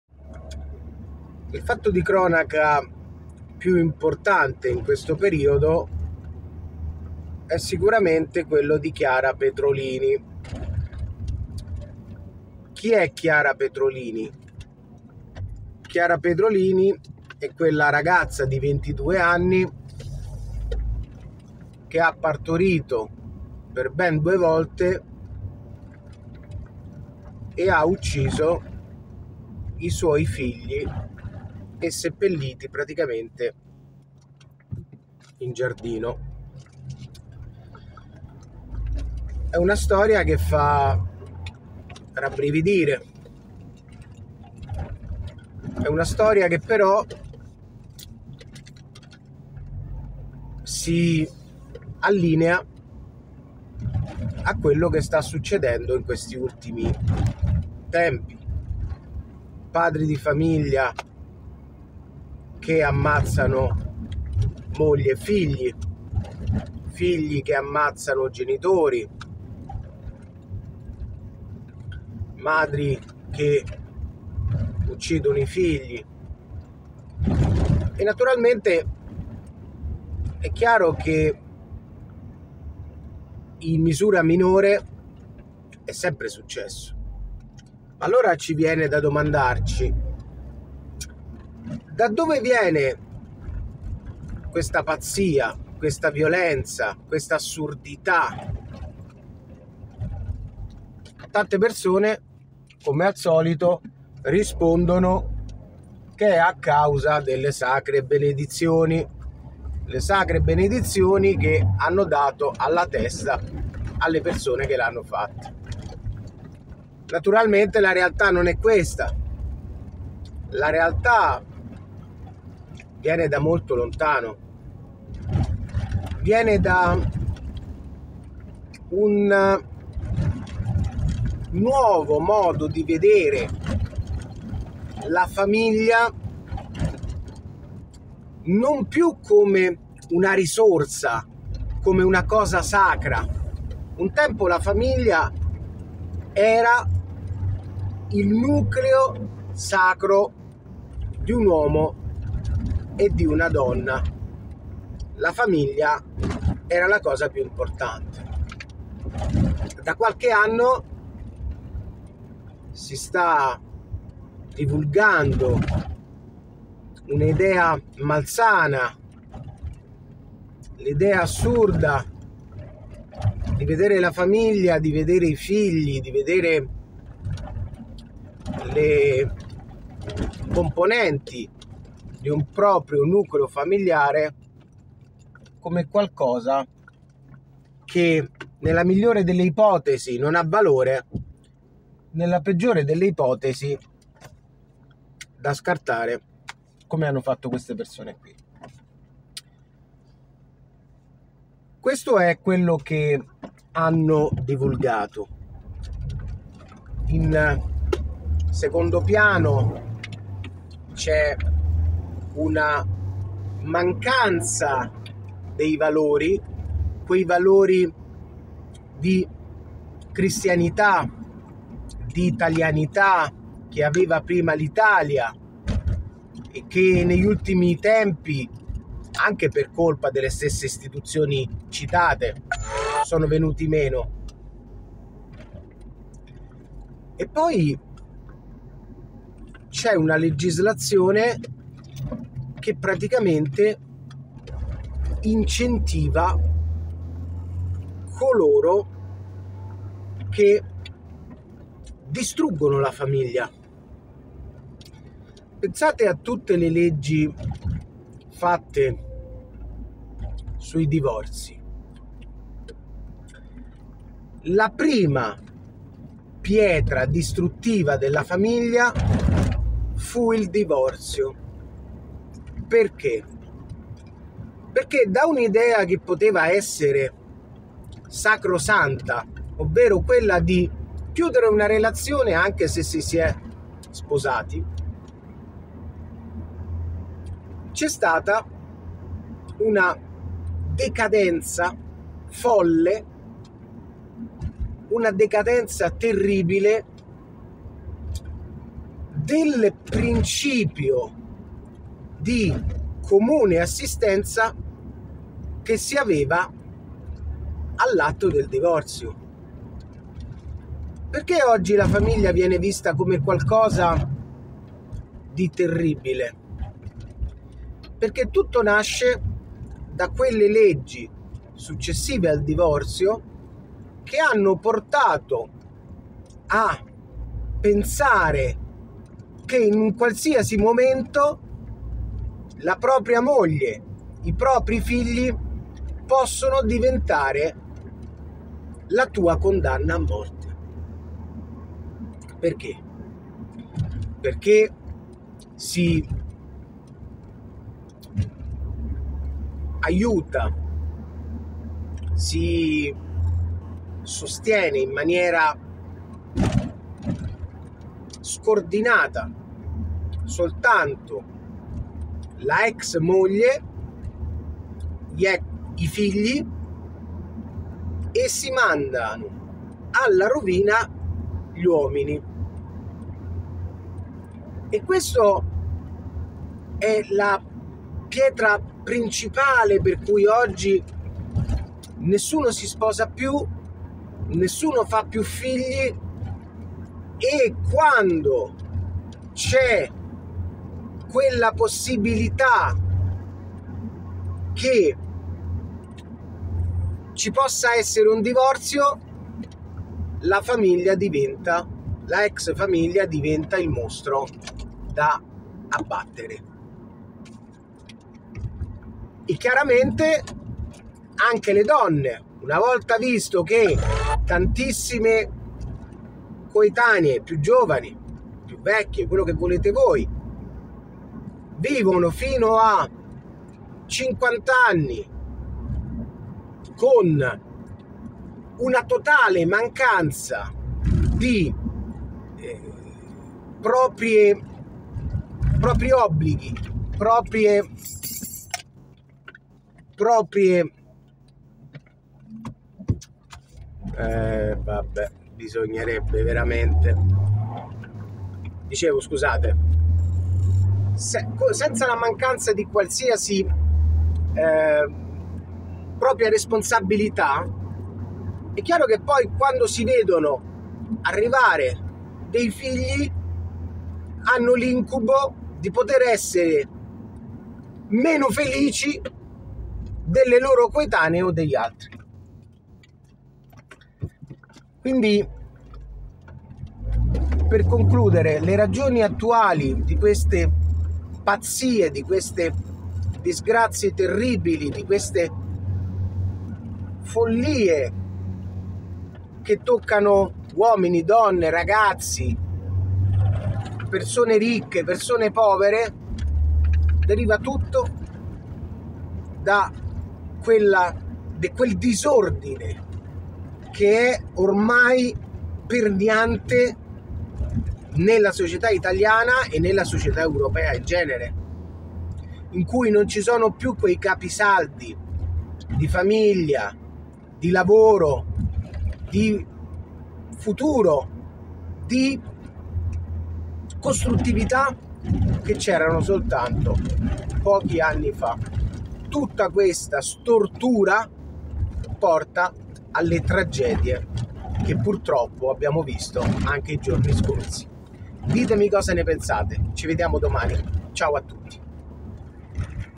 CHIARA PETROLINI, E LE RAGIONI DEL DISPREZZO ATTUALE DELLA VITA ‼️
Il caso di cronaca dichiara petrolini, la mamma che ha ucciso i due bambini, ci porta a riflettere attentamente sulla mancanza di valori di questo momento storico